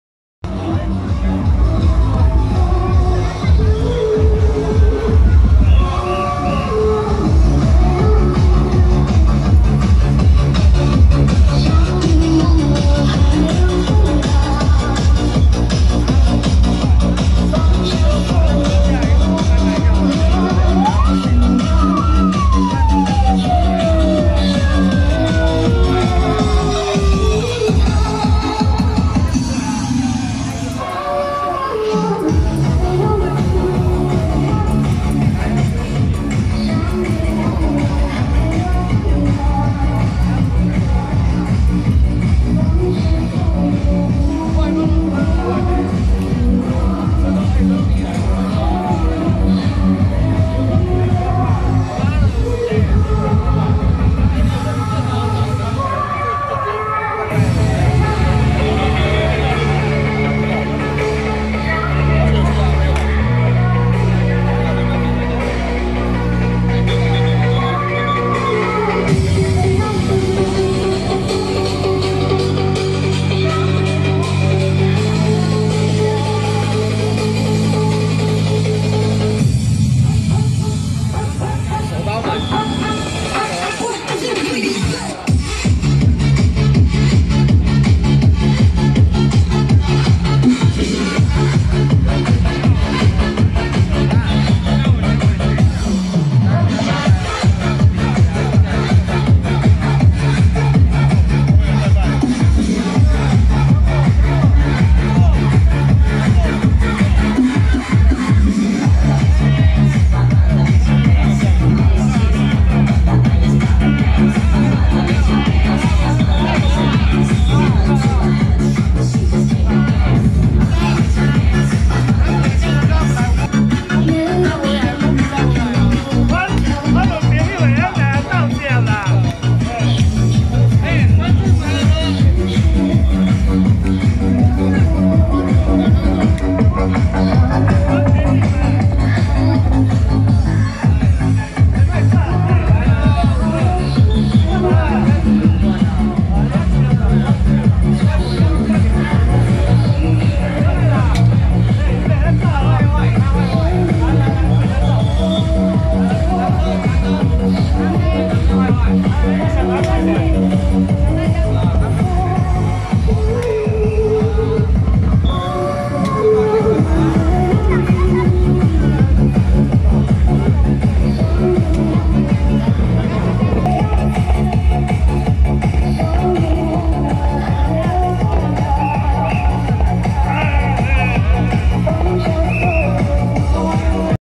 【音速震團 源聲汽車音響車隊】~110楊梅北極玄天宮玄天上帝南巡進香回駕遶境
音速震團 源聲汽車音響車隊